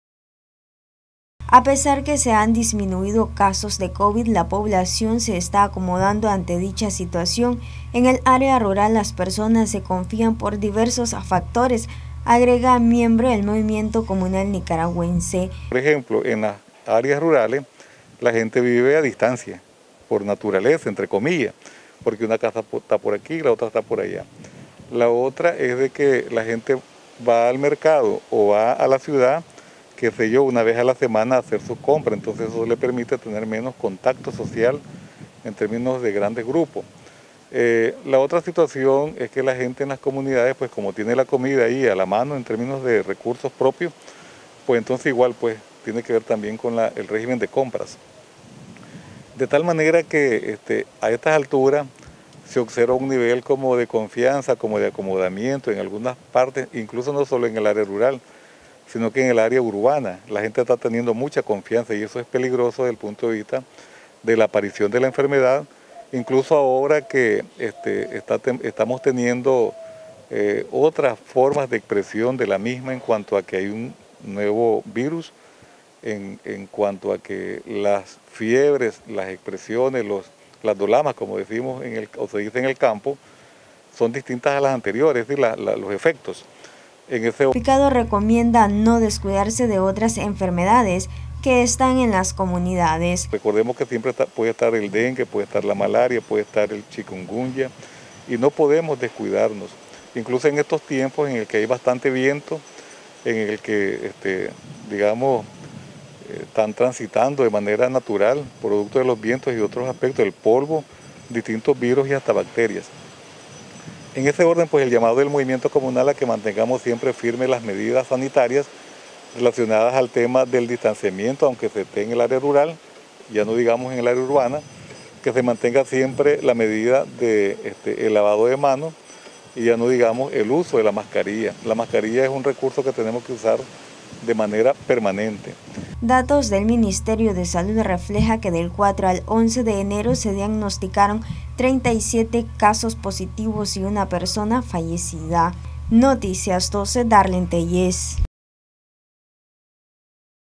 Comunidades rurales se han "confiado" ante la pandemia del covid-19
Noticias12Nicaragua Movimiento Comunal Nicaragüense percibe en algunas comunidades rurales al igual que en las ciudades confianza y acomodamiento ante la pandemia, situación que resulta peligrosa.